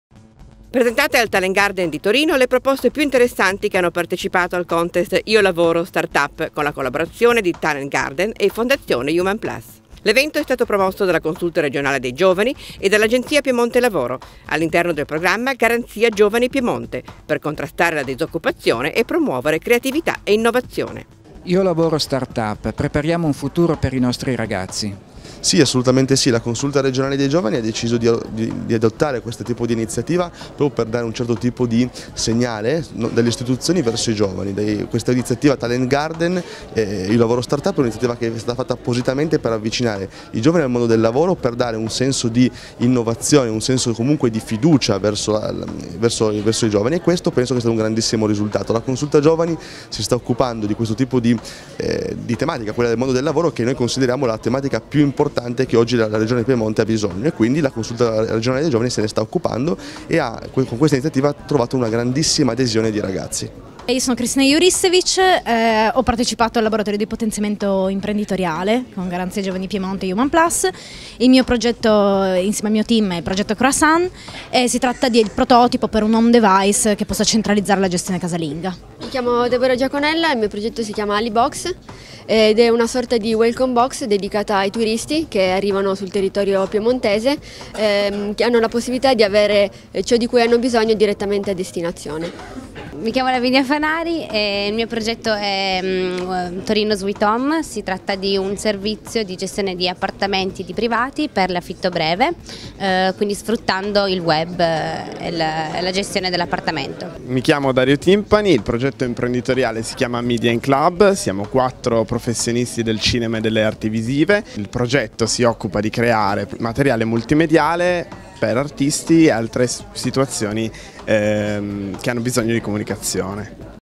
Contest Iolavoro Start up
Presentate, al Talent Garden di Torino, le proposte più interessanti che hanno partecipato al Contest IoLavoro Start up con la collaborazione di Talent Garden e Fondazione Human+. L’evento è stato promosso dalla Consulta regionale dei Giovani e dall’Agenzia Piemonte Lavoro, all’interno del programma Garanzia Giovani Piemonte, per contrastare la disoccupazione e promuovere creatività e innovazione.

intervista a

Alessandro Benvenuto, consigliere delegato alla Consulta Giovani

Cristina Iurissevich progetto CROISSANT
Deborah Giaconella progetto ALLY BOX –WELCOME BOX
Lavinia Fanari progetto TORINO SWEET HOME
Dario Timpani progetto MEDIENCLUB: VIDEO E CROSS MEDIA